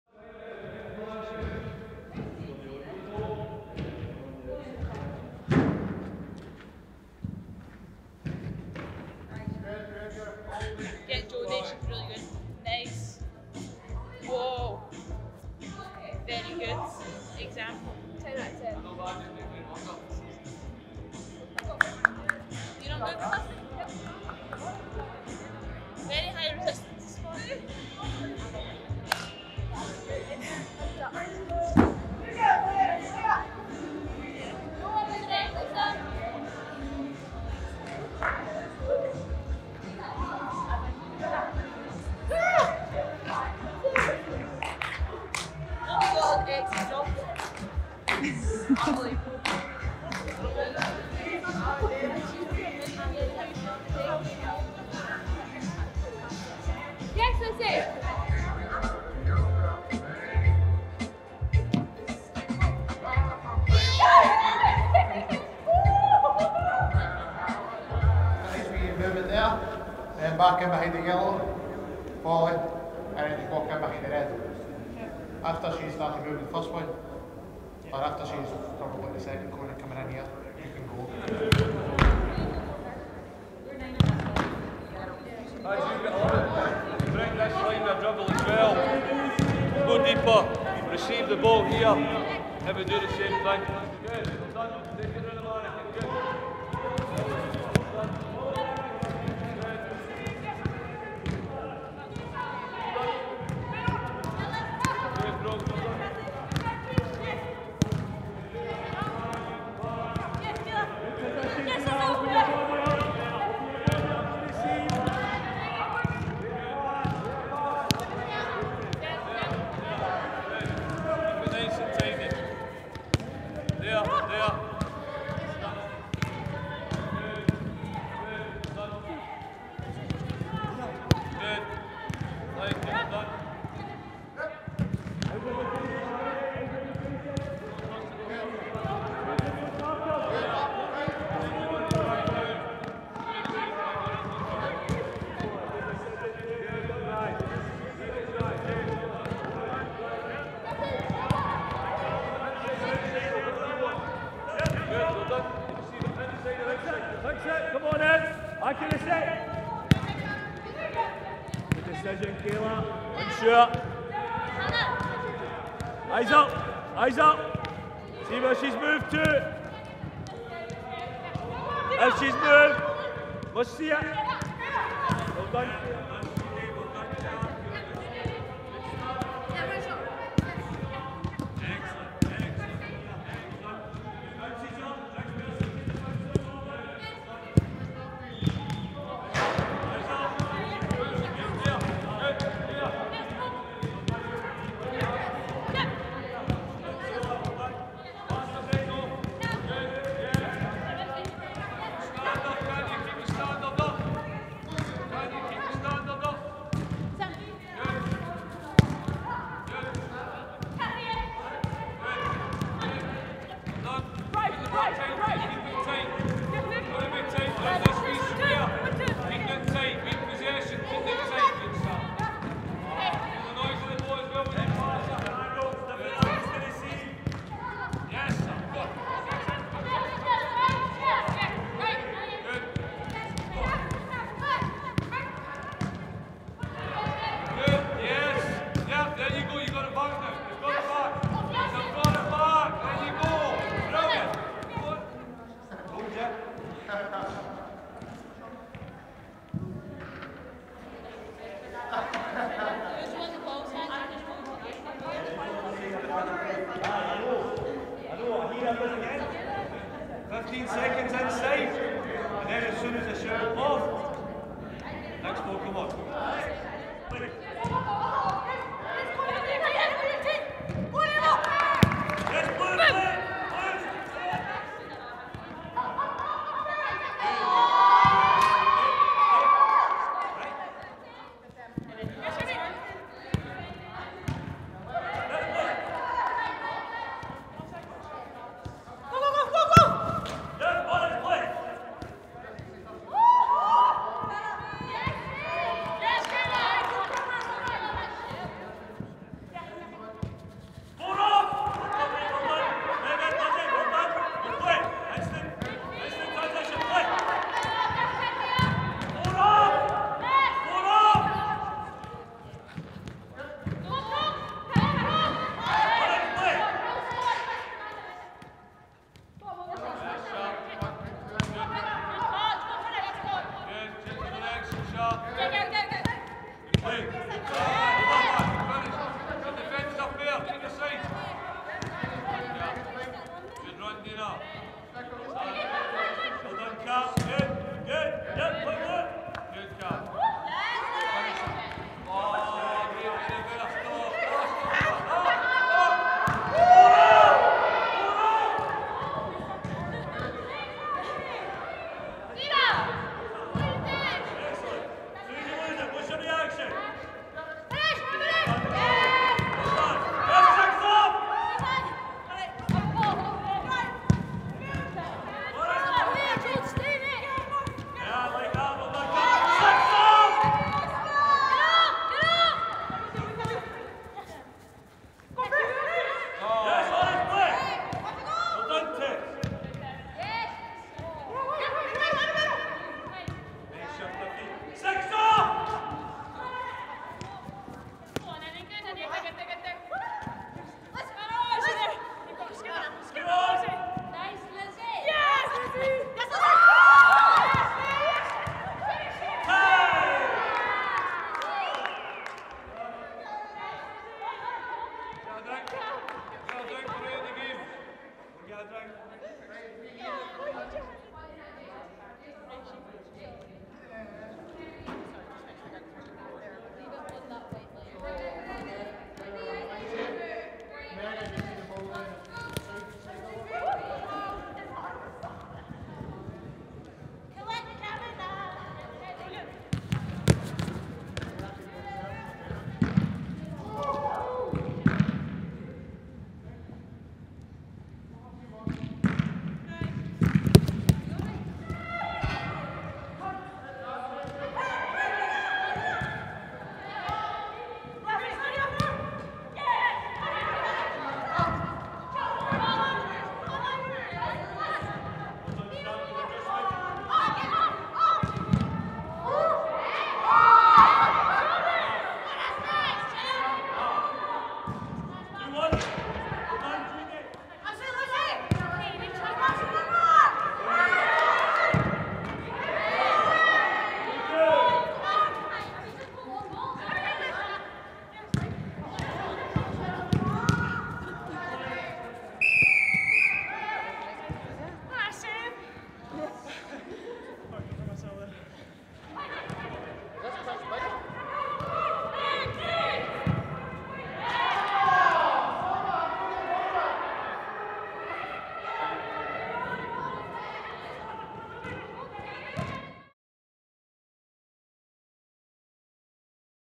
TRAINING GROUND | All Eyes on Aberdeen | 16 Dec 2022
Rangers Women work hard indoors as they return to league action this Sunday to face Aberdeen in the last game of the year.